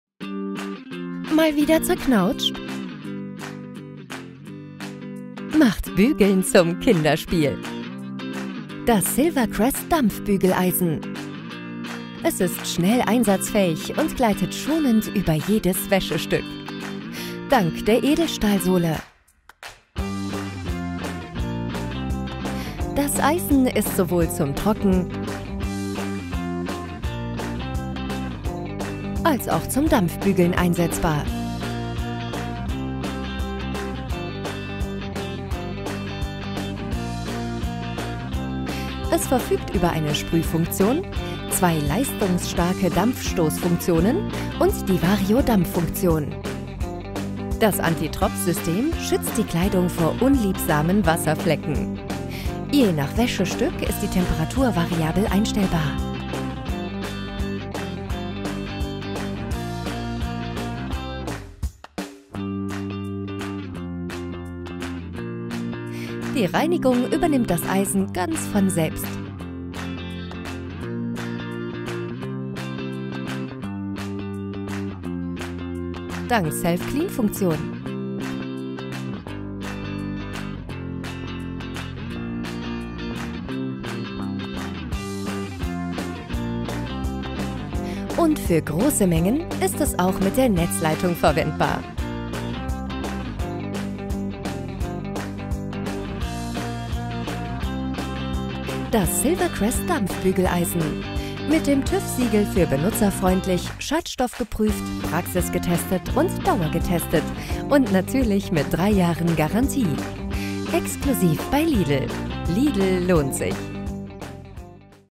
Produktvideo | Silvercrest kabelloses Dampfbügeleisen | Lidl lohnt sich
Mit dem kabellosen Dampfbügeleisen von Silvercrest könnt ihr "Bye Bye" 👋🏼  zu zerknitterter Wäsche sagen.

Produktdetails:

- Dampfbügel-, Dampfstoß-, Sprüh- und Trockenbügelfunktion
- Mit Antitropfsystem, Antikalk- und Selbstreinigungsfunktion „SelfClean“
- 2 leistungsstarke Dampffunktionen:
    - Dampfstoß (horizontal und vertikal) und variabel einstellbarer -
    - Permanentdampf (Vario-Dampf)
- Tankfüllmenge: ca. 360 ml (inklusive Einfüllbecher)